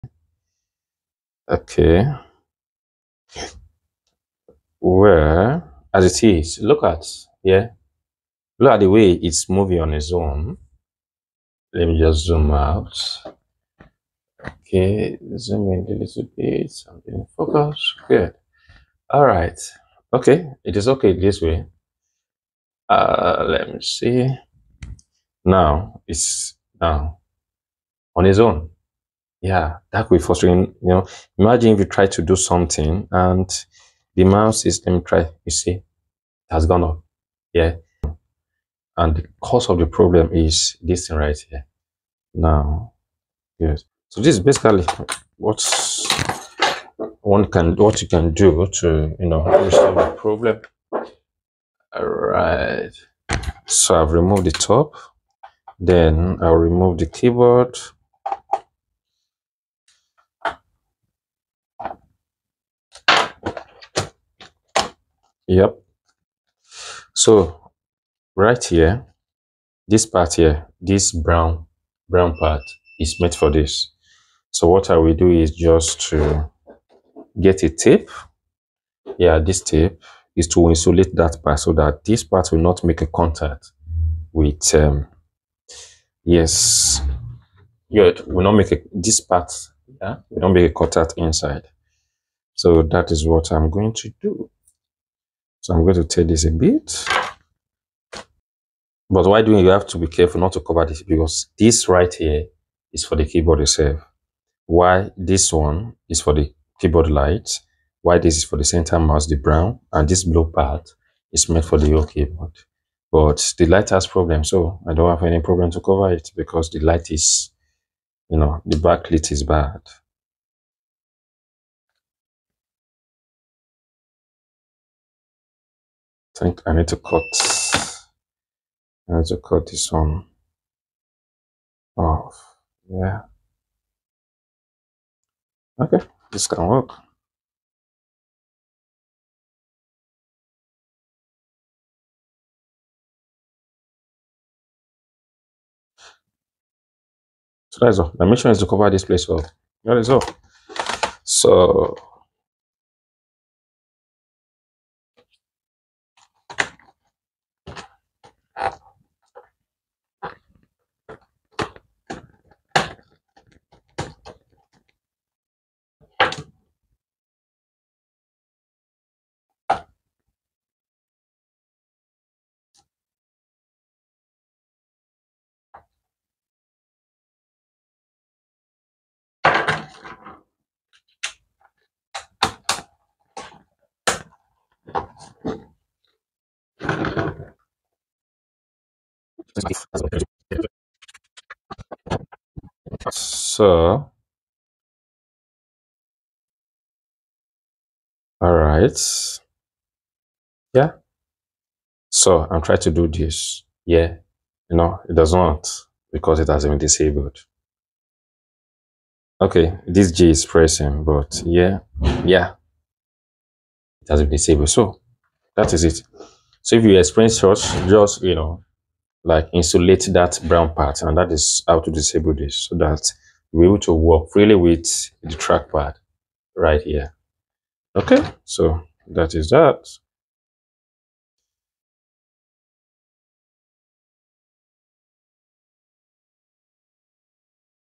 Dell Latitude E6410 Cursor Misbehaving Fixed!
Cursor moves on its own without touch, thereby making it challenging to use the PC.

Watch and know the cause and how it was fixed.